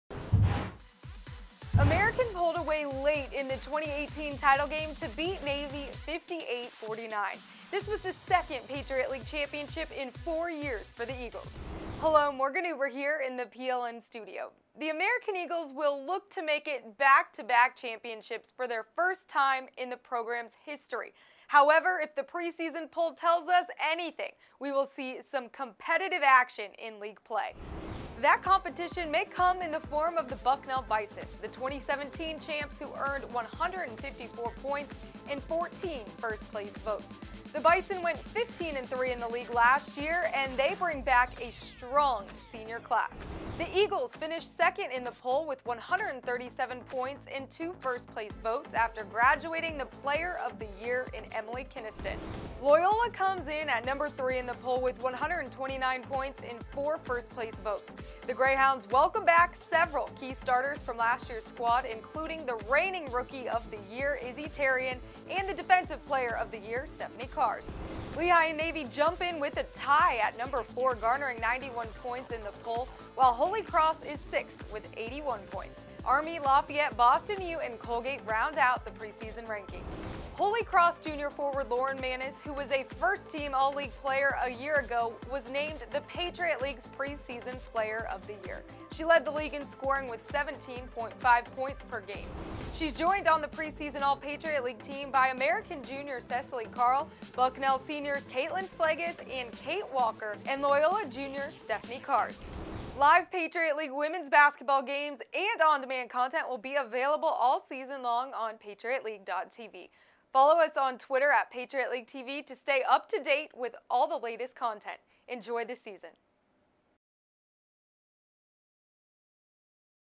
Patriot League announces 2018-19 women's basketball preseason poll and awards
The 2017 Patriot League champion Bucknell Bison earned the top spot in the Patriot League Women's Basketball Preseason Poll just ahead of the 2018 champion American Eagles. Both schools combined for three of five preseason All-League players, while Holy Cross boasts the preseason player of the year in Lauren Manis.